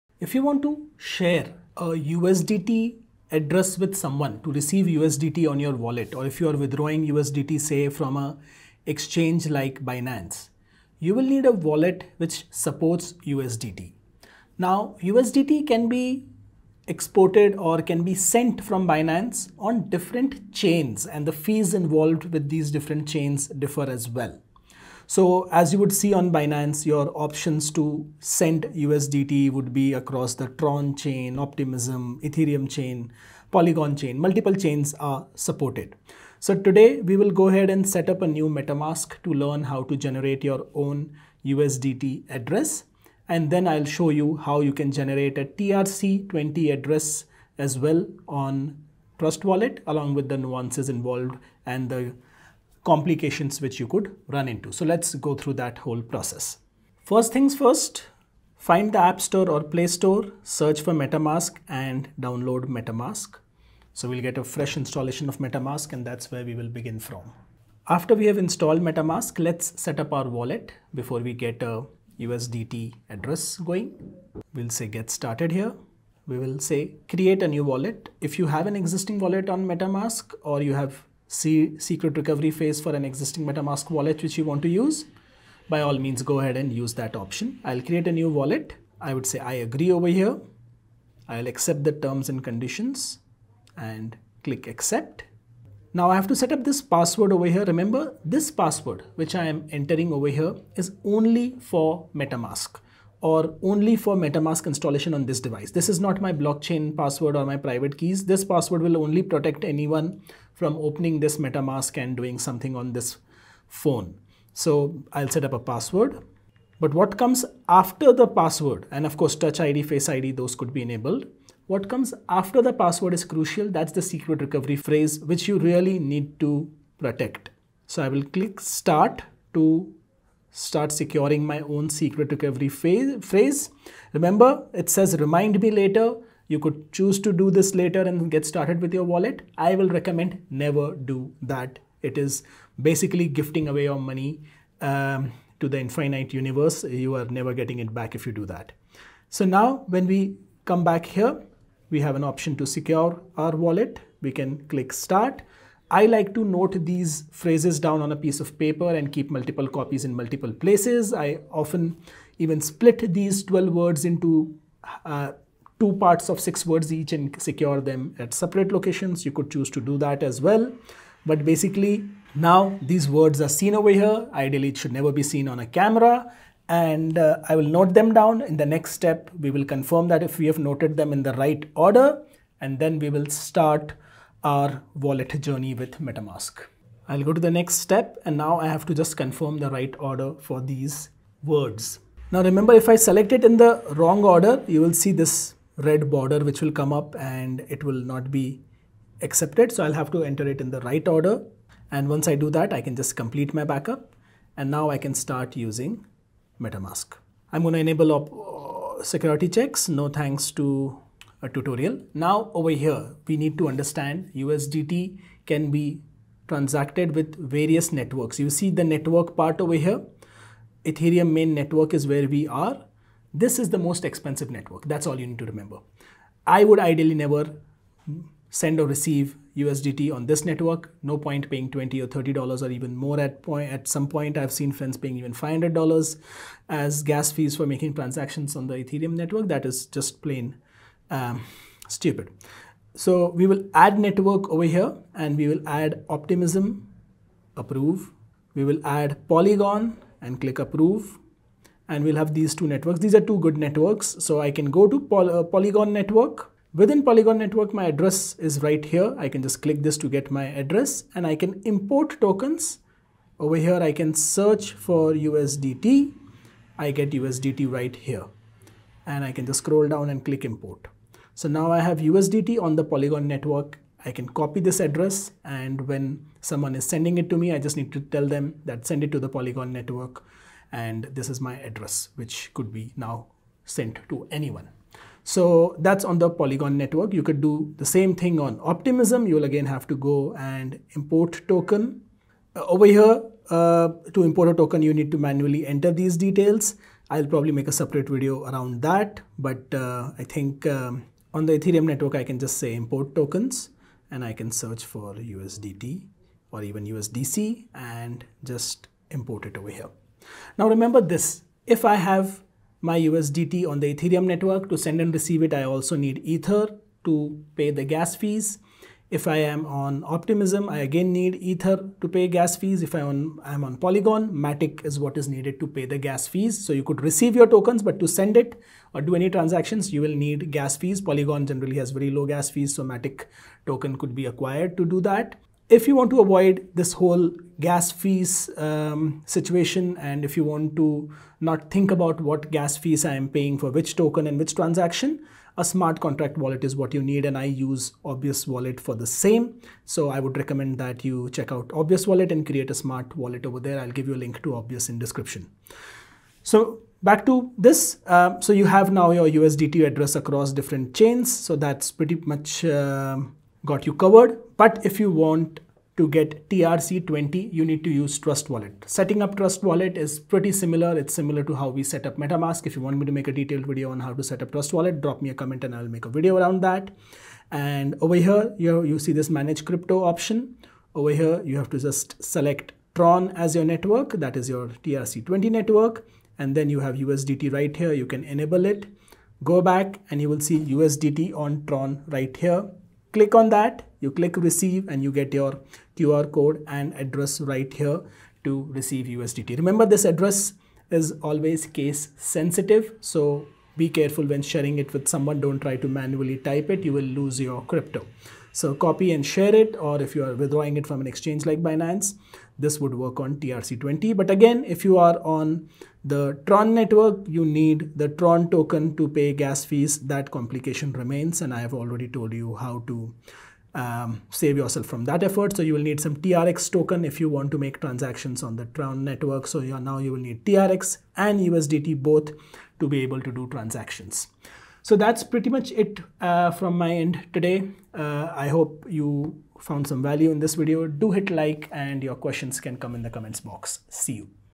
Get USDT Wallet Address on TRC20, Polygon, Optimism and Other Chains - USDT Wallet
Want to know what is the USTD address on your preferred blockchain? Watch this video. I've shown where you can find the USDT wallet address in this video. So whether you are on Polygon, Optimism, or other chain, you will be able to see the address easily.